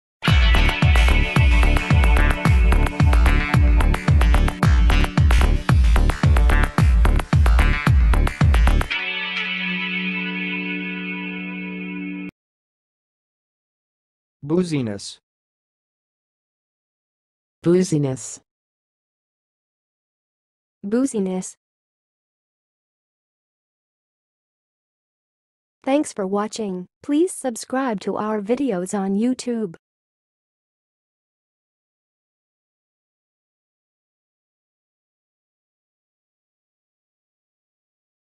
How to say "booziness"! (High Quality Voices)
Watch in this video how to say and pronounce "booziness"!